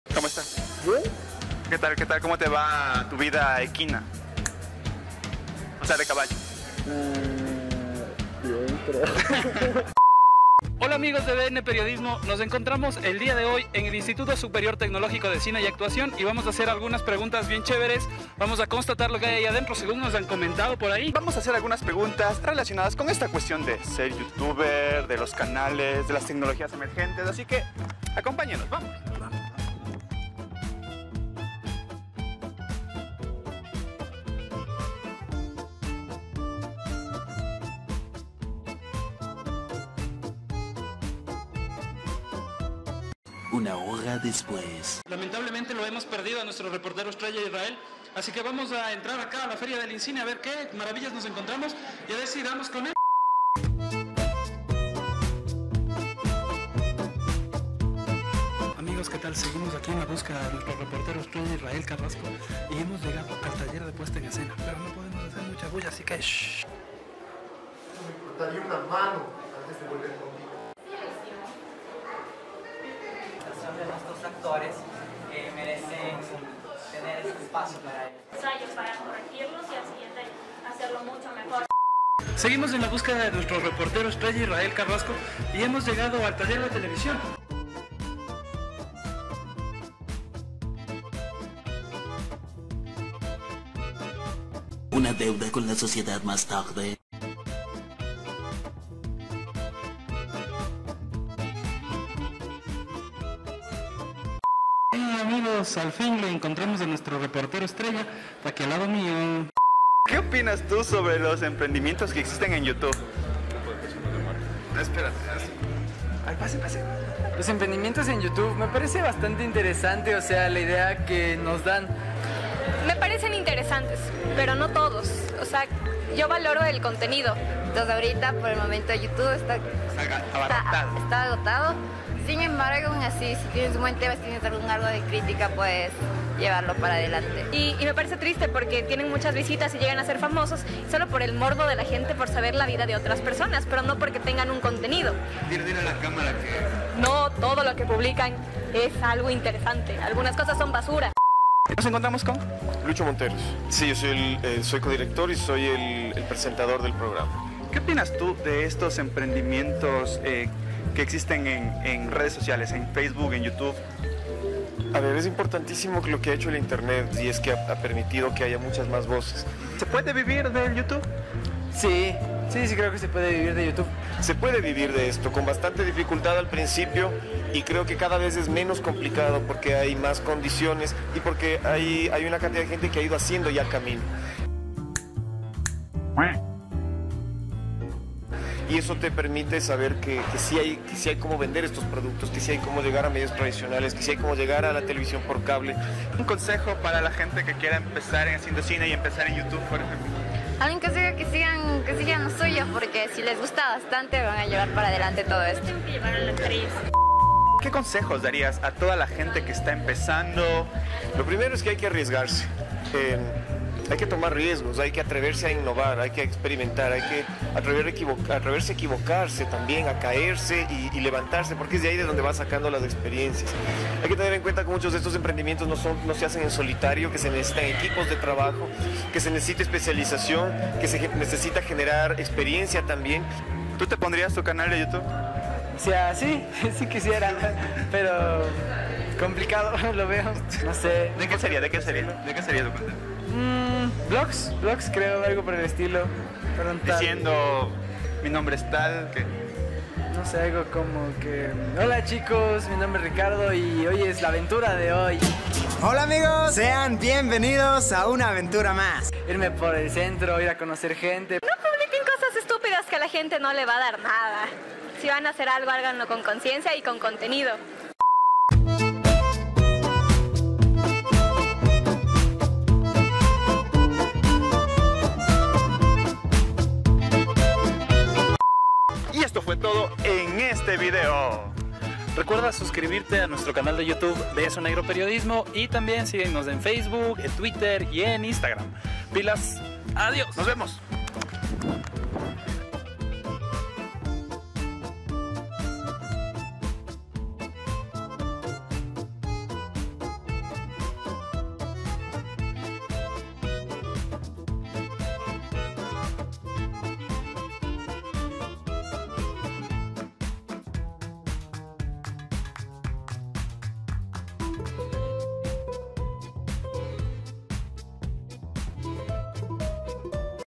La escuela de EnchufeTV 🎬 | Emprendimientos digitales | INCINE | Beso Negro - Periodismo
En este vídeo visitamos la feria del Instituto de Cine y Actuación (INCINE), lugar donde estudiaron muchos miembros de enchufetv, para hacer preguntas sobre los emprendimientos digitales y mucho más.

Entrevistas: 1:50